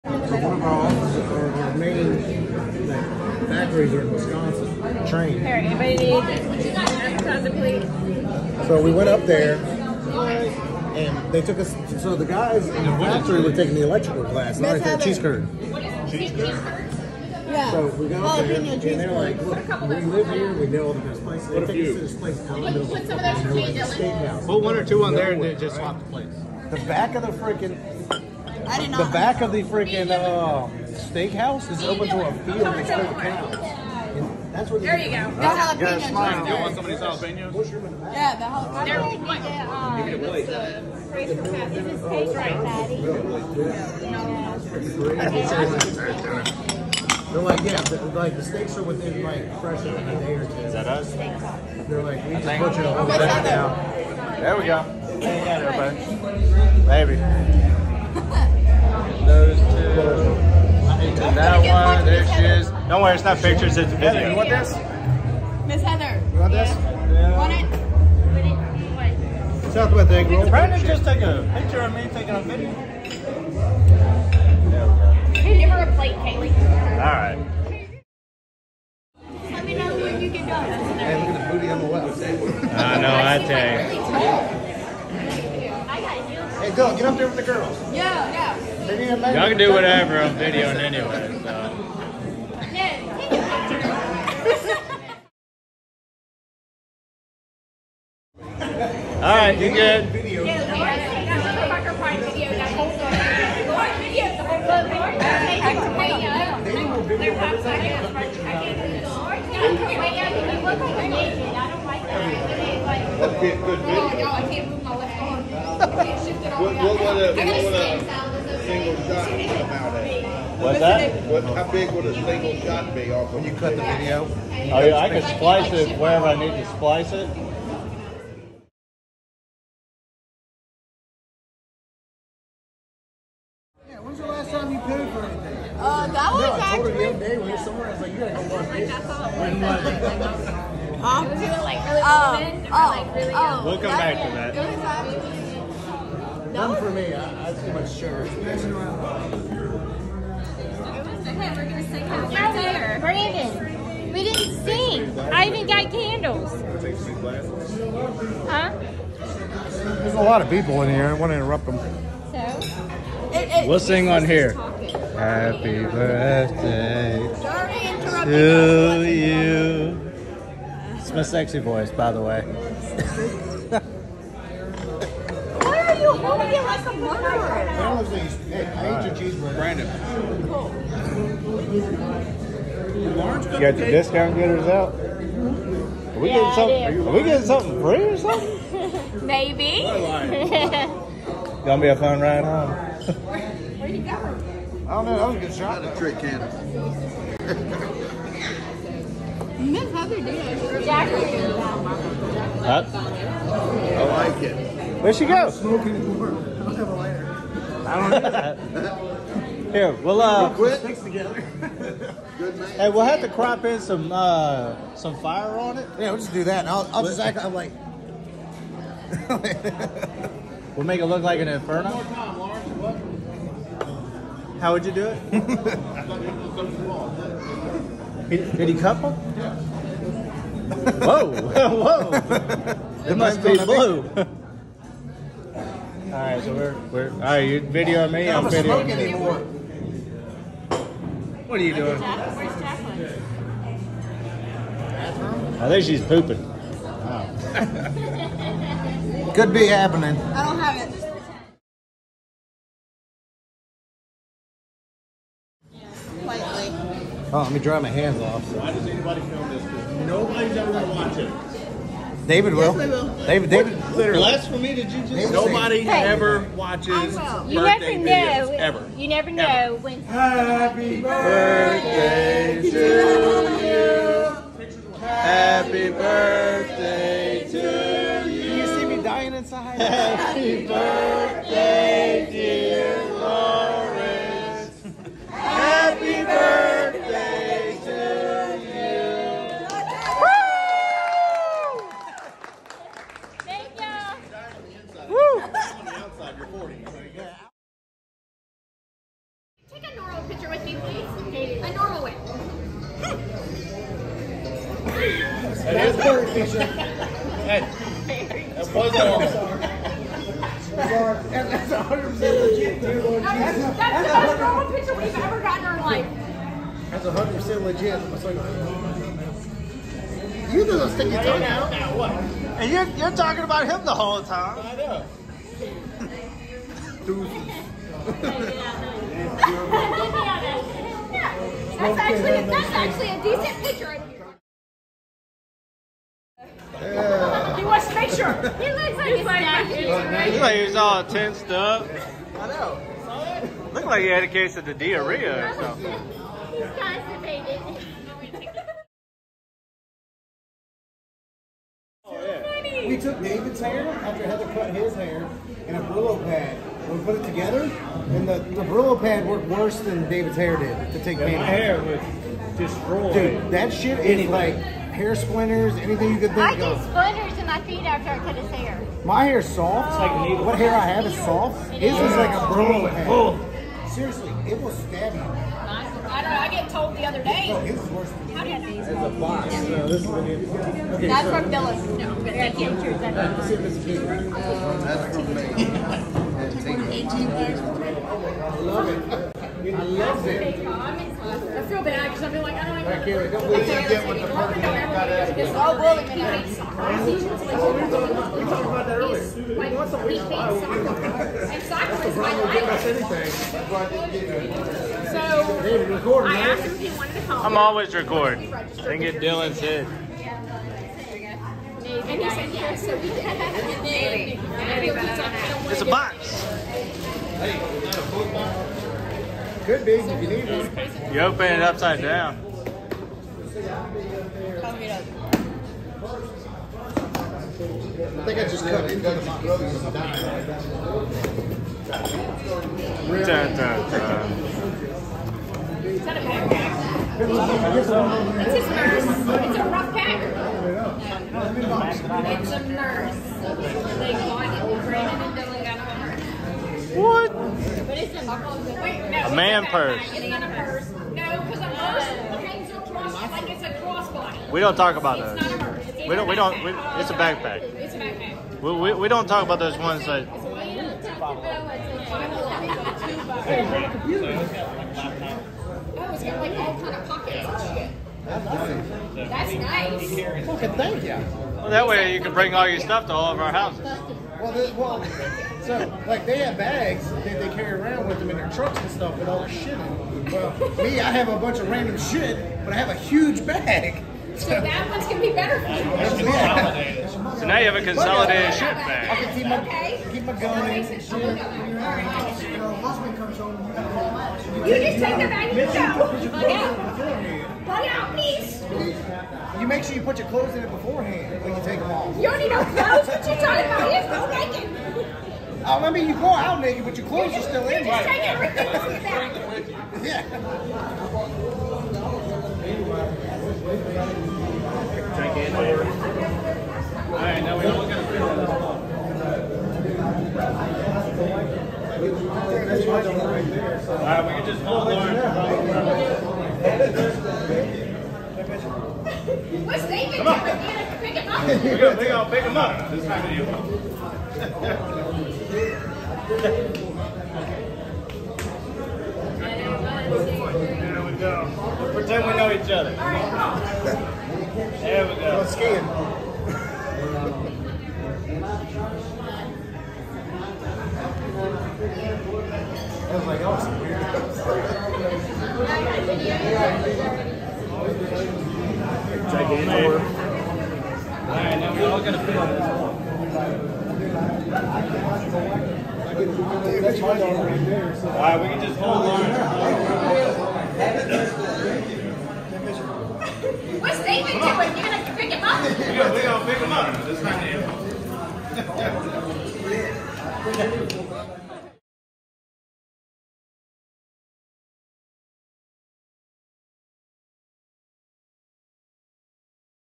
The BIG 40! Happy Birthday Lawrence Marchetta Like a “Brother from another Mother” 12/09/22
The BIG 40! Happy Birthday Lawrence Marchetta Like a “Brother from another Mother” 12/09/22 at Sully’s in Gulfport, MS

Lawrence Marchetta
Kayleigh Marchetta
Jacylyn Marchetta

David Gaines
Heather Gaines

Brandon Normand
Diana Normand

Dylan Murphy
           Murphy